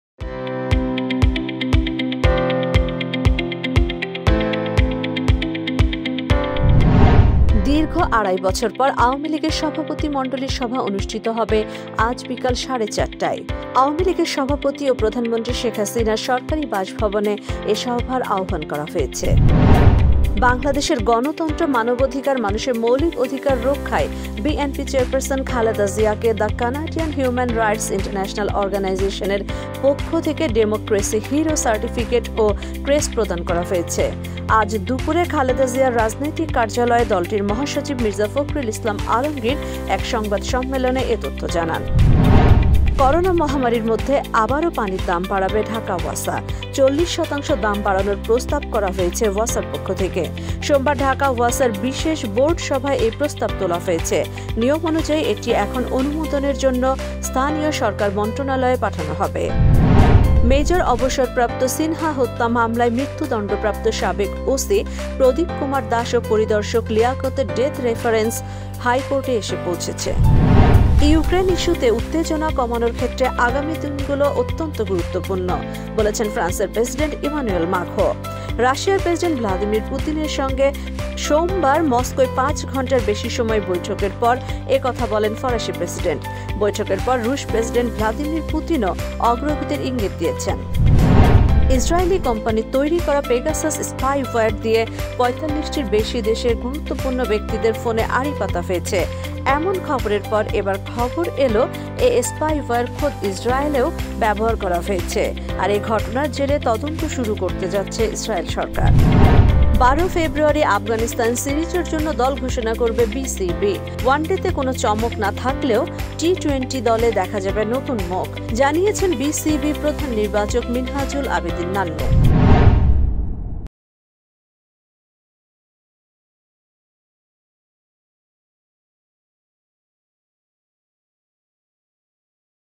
বিকালের নিউজ ফ্ল্যাশ (৮ফেব্রুয়ারি২০২২)
আড়াই বছর পর আওয়ামী লীগের সভাপতিমন্ডলীর সভা মঙ্গলবার

দীর্ঘ আড়াই বছর পর আওয়ামী লীগের সভাপতিমন্ডলীর সভা অনুষ্ঠিত হবে আজ বিকাল সাড়ে ৪টায়। আওয়ামী লীগের সভাপতি ও প্রধানমন্ত্রী শেখ হাসিনার সরকারি বাসভবনে এ সভার আহ্বান করা হয়েছে।

'ডেমোক্রেসি হিরো' উপাধি পেলেন খালেদা

বাংলাদেশের গণতন্ত্র, মানবাধিকার, মানুষের মৌলিক অধিকার রক্ষায় বিএনপি চেয়ারপারসন খালেদা জিয়াকে 'দি কানাডিয়ান হিউম্যান রাইটস ইন্টারন্যাশনাল অর্গানাইজেশন' এর পক্ষ থেকে 'ডেমোক্রেসি হিরো' সার্টিফিকেট ও ক্রেস্ট প্রদান করা হয়েছে। মঙ্গলবার (৮ ফেব্রুয়ারি) দুপুরে খালেদা জিয়ার রাজনৈতিক কার্যালয়ে দলটির মহাসচিব মির্জা ফখরুল ইসলাম আলমগীর সংবাদ সম্মেলনে এ তথ্য জানান।

আরেক দফা পানির দাম বাড়াবে ঢাকা ওয়াসা

করোনা মহামারির মধ্যে আবারও পানির দাম বাড়াবে ঢাকা ওয়াসা। ৪০ শতাংশ দাম বাড়ানোর প্রস্তাব করা হয়েছে। সোমবার (৭ জানুয়ারি) ঢাকা ওয়াসার বিশেষ বোর্ড সভায় এই প্রস্তাব তোলা হয়েছে। নিয়ম অনুযায়ী এটি এখন অনুমোদনের জন্য স্থানীয় সরকার মন্ত্রণালয়ে পাঠানো হবে।

সিনহা হত্যা: ওসি প্রদীপের ডেথ রেফারেন্স হাইকোর্টে
মেজর (অব.) সিনহা হত্যা মামলায় মৃত্যুদণ্ডপ্রাপ্ত সাবেক ওসি প্রদীপ কুমার দাশ ও পরিদর্শক লিয়াকতের ডেথ রেফারেন্স (মামলার যাবতীয় নথি) হাইকোর্টে এসে পৌঁছেছে।

ইউক্রেন ইস্যুতে পুতিন-ইমানুয়েল বৈঠক

ফ্রান্সের প্রেসিডেন্ট ইমানুয়েল মাখোঁ বলেছেন, ইউক্রেন ইস্যুতে উত্তেজনা কমানোর ক্ষেত্রে আগামী দিনগুলো অত্যন্ত গুরুত্বপূর্ণ। রাশিয়ার প্রেসিডেন্ট ভ্লাদিমির পুতিনের সঙ্গে গতকাল (সোমবার) মস্কোয় পাঁচ ঘণ্টার বেশি সময় বৈঠকের পর এ কথা বলেন ফরাসি প্রেসিডেন্ট। বৈঠকের পর রুশ প্রেসিডেন্ট ভ্লাদিমির পুতিনও অগ্রগতির ইঙ্গিত দিয়েছেন।

পেগাসাস নিয়ে তদন্ত করবে ইসরায়েল

ইসরায়েলি কোম্পানির তৈরি করা পেগাসাস স্পাইওয়্যার দিয়ে ৪৫টির বেশি দেশে গুরুত্বপূর্ণ ব্যক্তিদের ফোনে আড়ি পাতা হয়েছে এমন খরবের পর, এবার খবর এল এ স্পাইওয়্যার খোদ ইসরায়েলেও ব্যবহার করা হয়েছে। আর এ ঘটনার জেরে তদন্ত শুরু করতে যাচ্ছে ইসরায়েল।

আফগান সিরিজের জন্য বাংলাদেশ দল ঘোষণা ১২ ফেব্রুয়ারি

১২ই ফেব্রুয়ারি আফগানিস্তান সিরিজের জন্য দল ঘোষণা করবে বিসিবি। ওয়ানডেতে কোন চমক না থাকলেও টি-টোয়েন্টি দলে দেখা যাবে নতুন মুখ। জানিয়েছেন বিসিবির প্রধান নির্বাচক মিনহাজুল আবেদিন নান্নু।